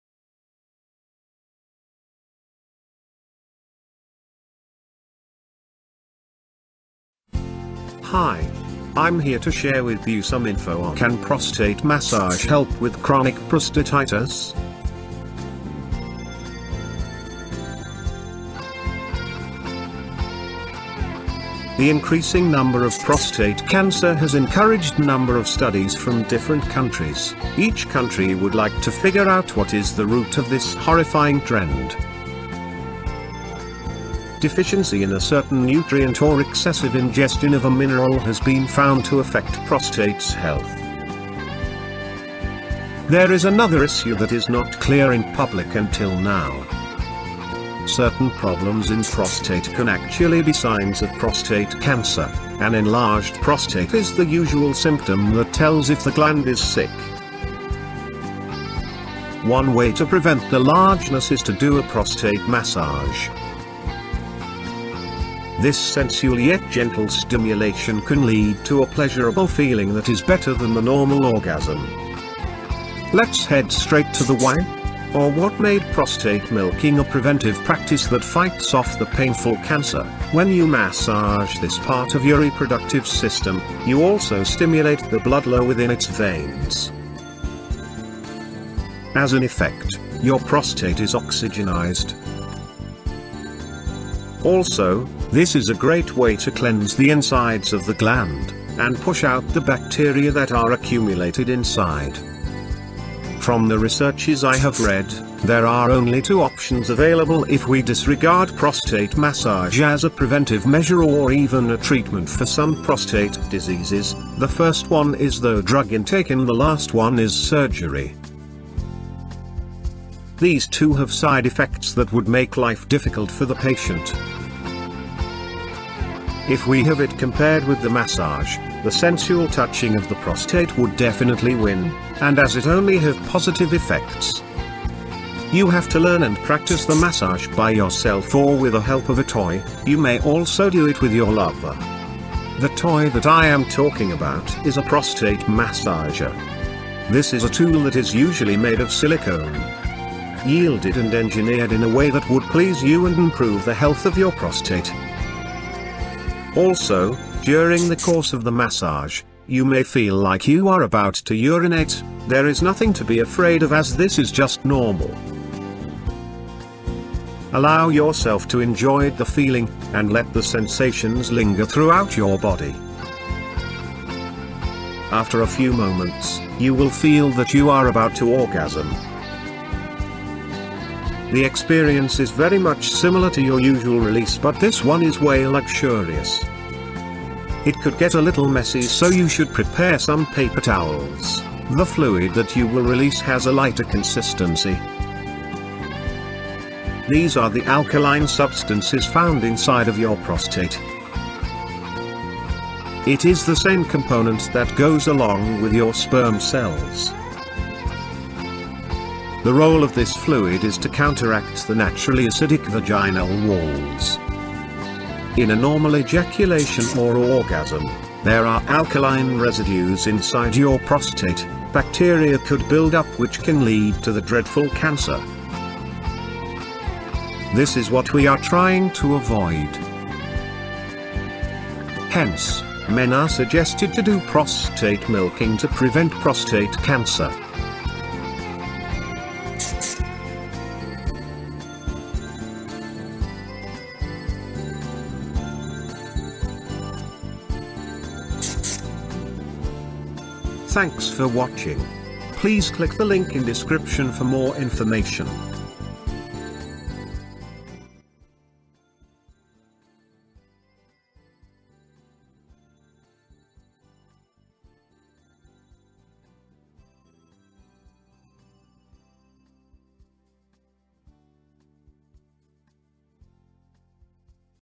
Prostate Massage For Chronic Prostatitis-Best Cure?
Can prostate massage help with chronic prostatitis? to find out how to perform this treatment from professional prostate massage therapists Ling Arturo and Jonas Sterling.

Men have been using this natural therapy as an alternative to conventional prostate treatments for many years.

If you are looking for other videos to watch, I think this is the best and funniest video on prostate massage

If you are interested in learning more about using prostate massage for chronic prostatitis then I would suggest checking out the website recommended in the video.